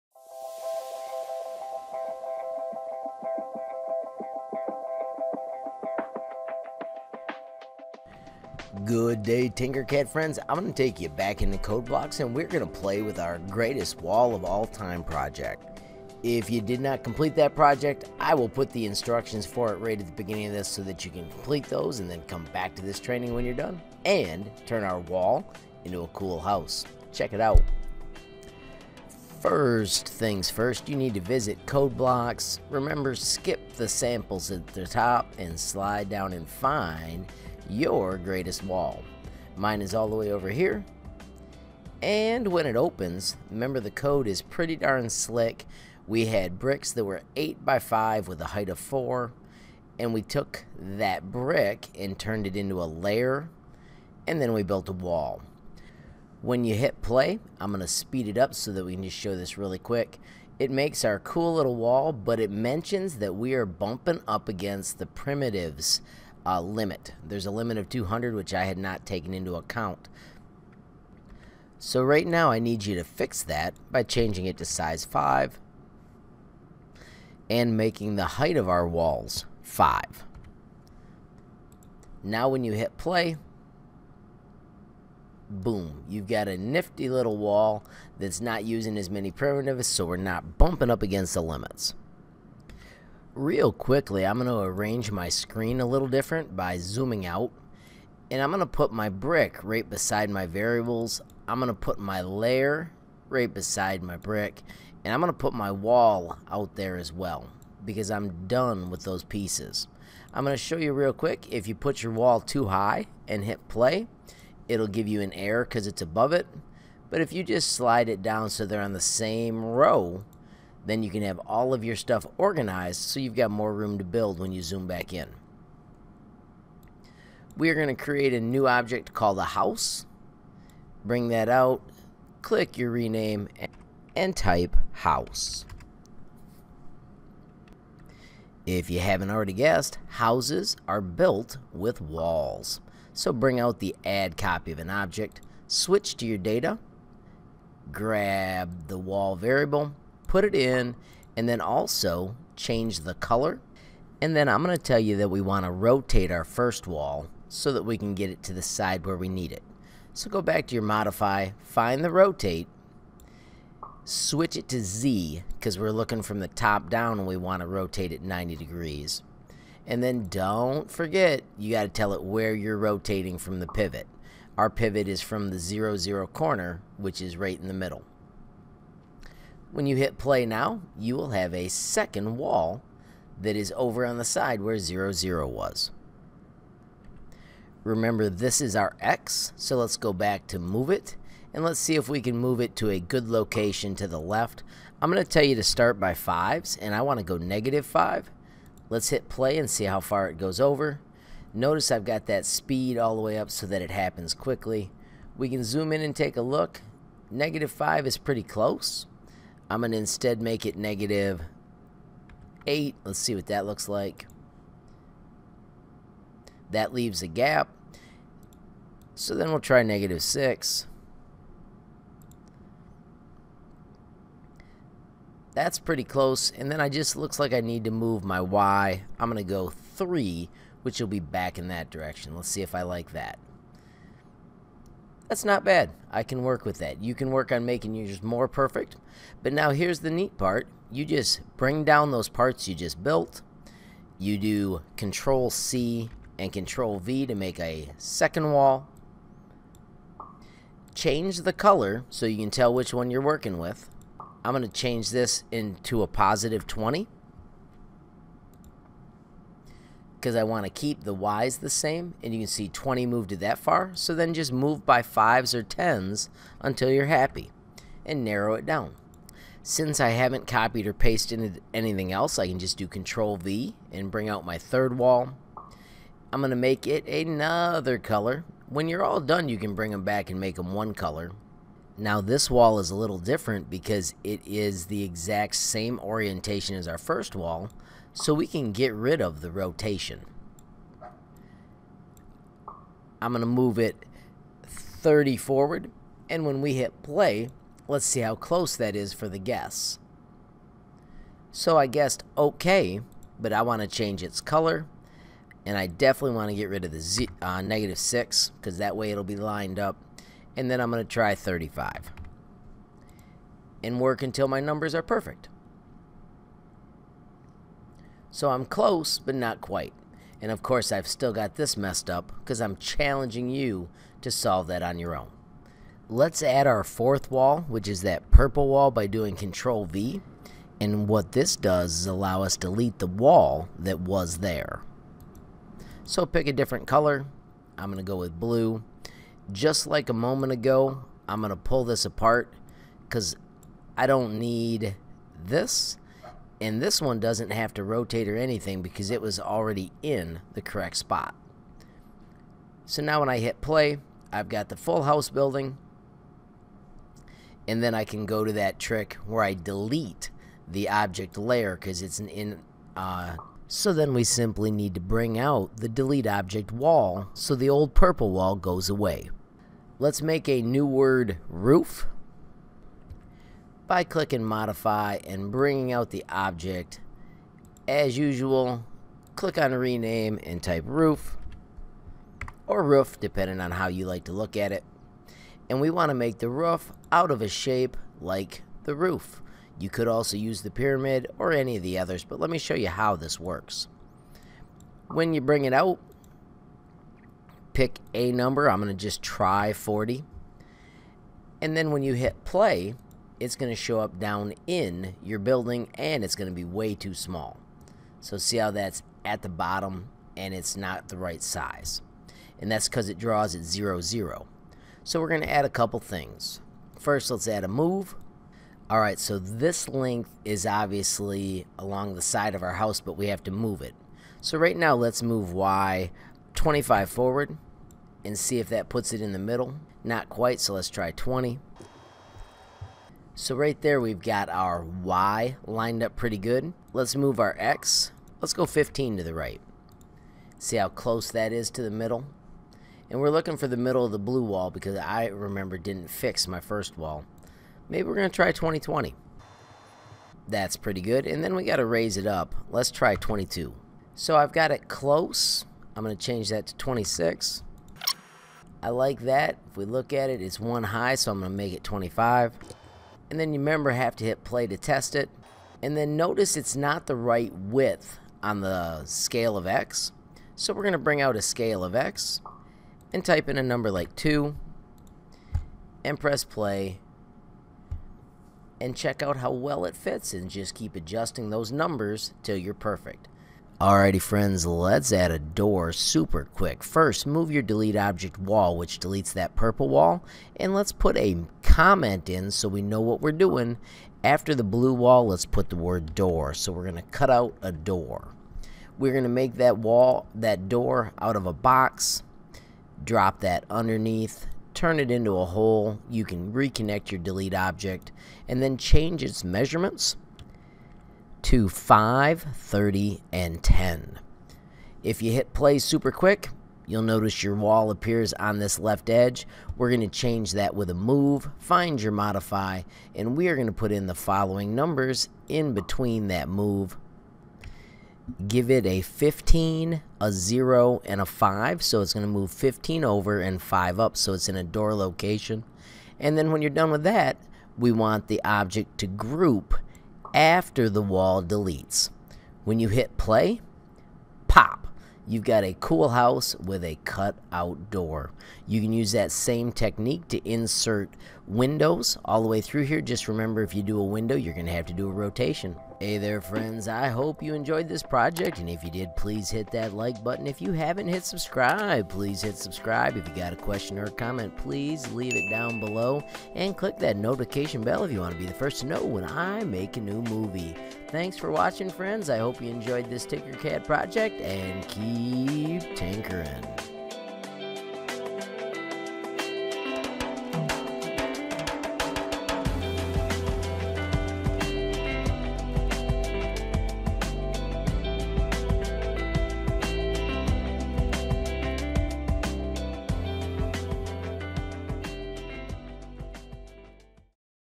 Let there be HOUSE! A Codeblocks Creation
Learning to code is fun and fast with the Tinkercad Codeblocks! It is also really neat to see an animated rendering of your sweet design when you finish!